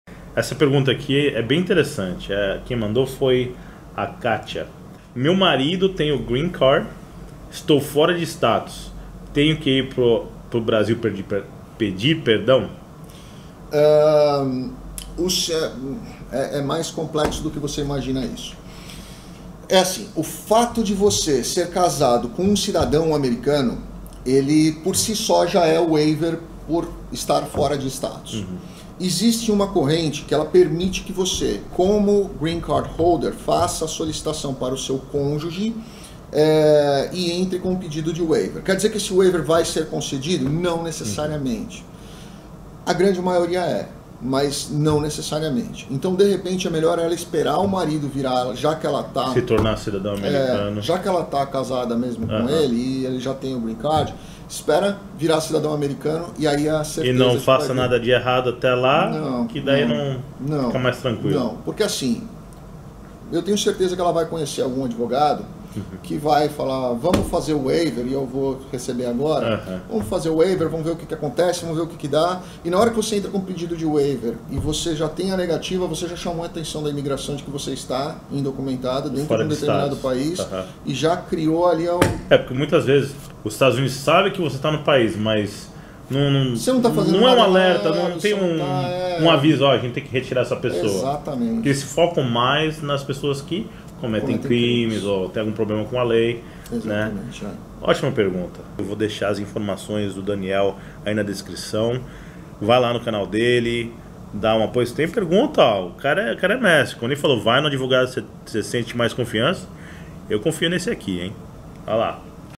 Posso receber Green Card do meu Marido nos Estados Unidos ou no Brasil?
Existem vários tipos de visto americanos:

⭐ VISTO EB-3: Visto para trabalhadores profissionais qualificados.
⭐ VISTO EB-5: Visto para investidores
⭐ VISTO L-1: Visto para empresários atuarem nos Estados Unidos
⭐ VISTO P: Vistos para atletas e vistos para artistas